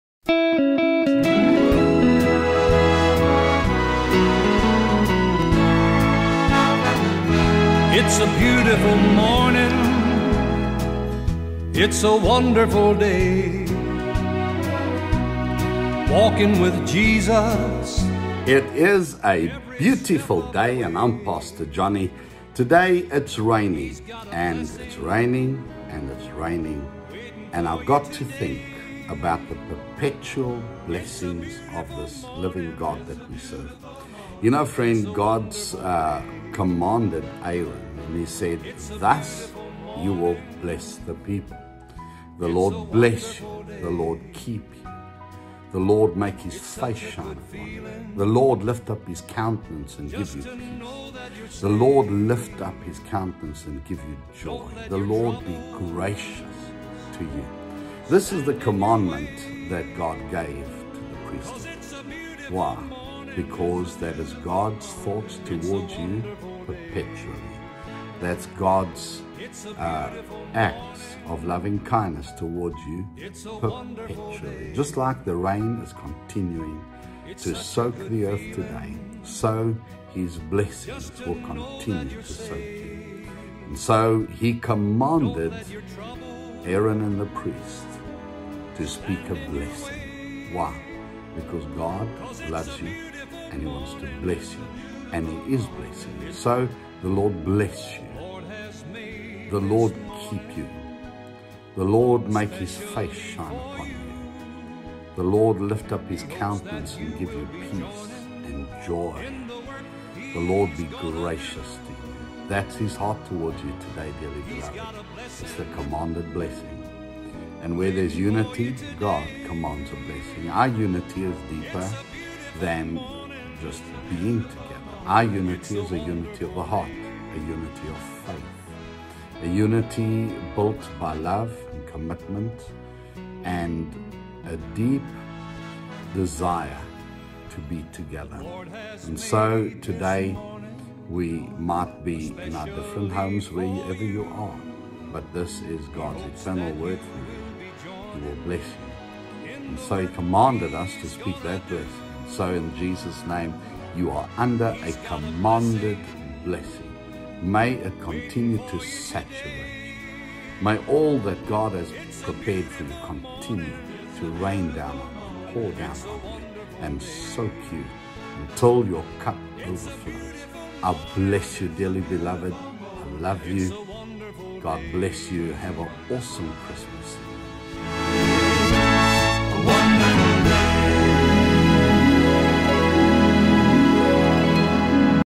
It's a Beautiful Day | The Blessing |10 November 2021
Start your morning with this encouraging word by Apostle Johny Grobler.

“The Lord bless you and keep you; the Lord make his face shine on you and be gracious to you; the Lord turn his face toward you and give you peace.” - Numbers 6:24-26

New to DCC South?

Because of your support, no matter the amount, someone will hear about Jesus Christ for the very first time today.